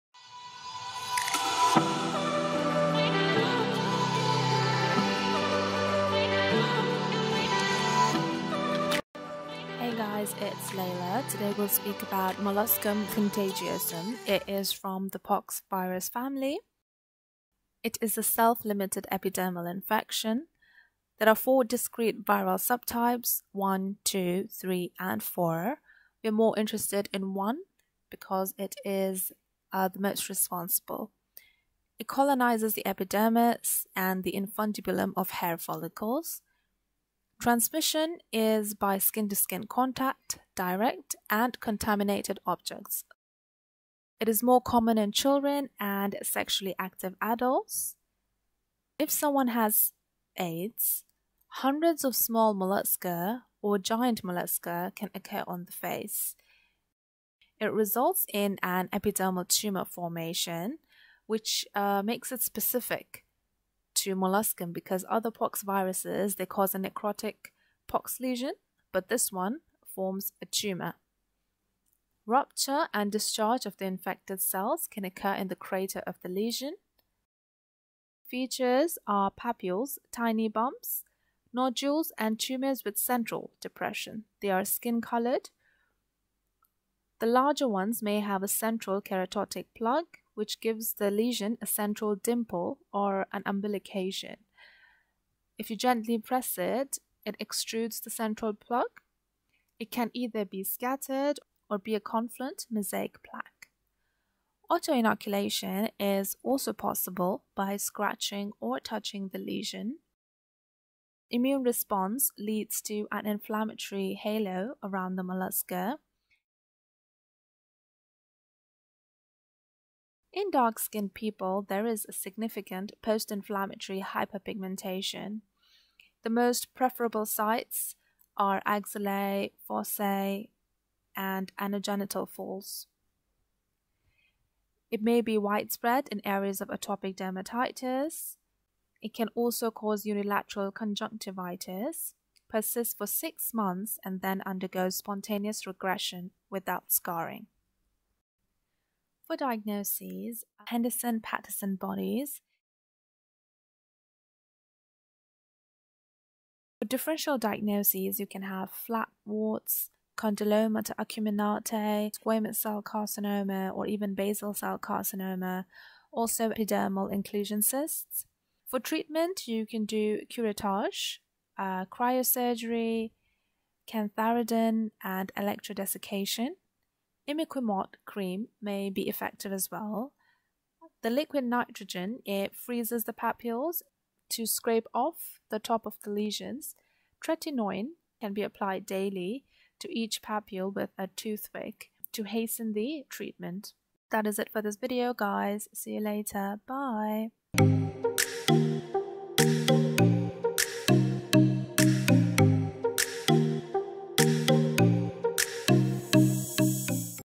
Molluscum Contagiosum
In this video I speak about the disease, course, features, treatment etc.